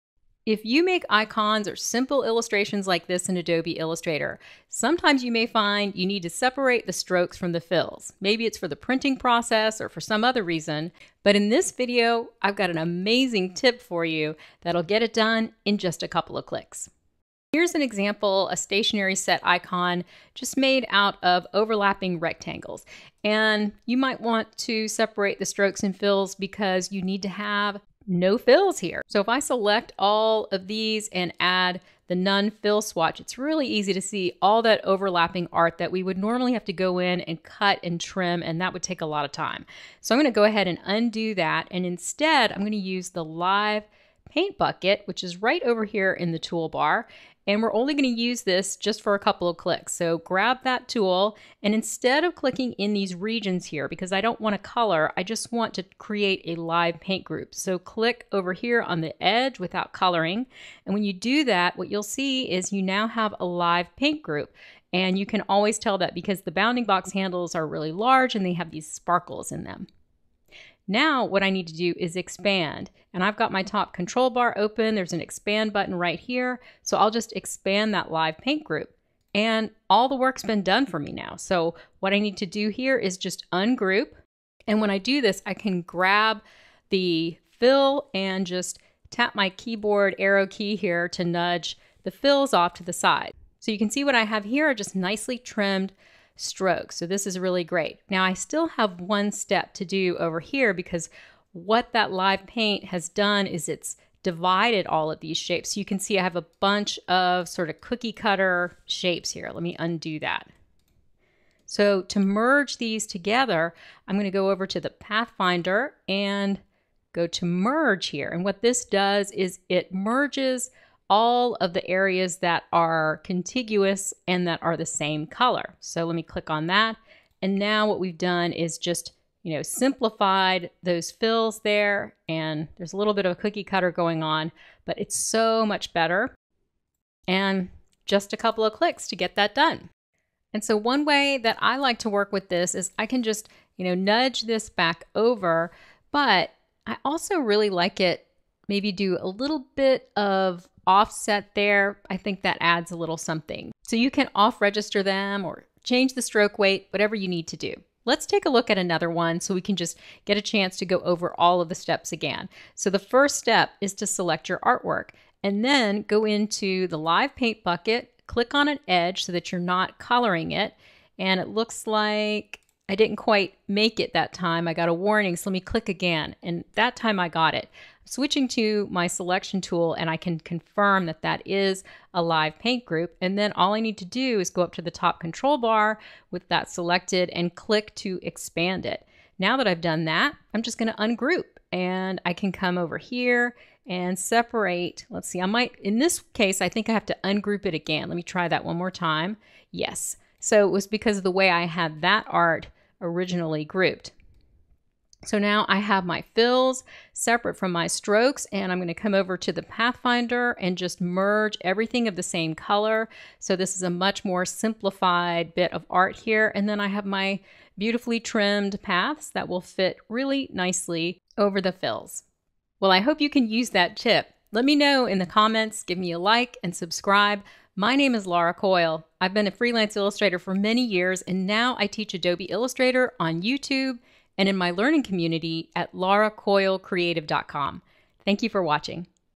How to Separate Strokes from Fills, Making Icons in Adobe Illustrator
When building icons from shapes, the overlapping art may need to be trimmed at some point in your process. In this video, I show you an amazing trick in Adobe Illustrator,  to trim your art while separating the strokes from the fills in just a few clicks. A major time-saver!

↗️ Extra Tip: What if you use brushes or width profiles on your strokes? Live Paint will convert these to a default 1 pt. uniform stroke when you make the live paint group (you'll see a warning first). You can apply the brush or width profile again after you have expanded. Work on a copy, so you can keep the original brush settings.

✏️LINKS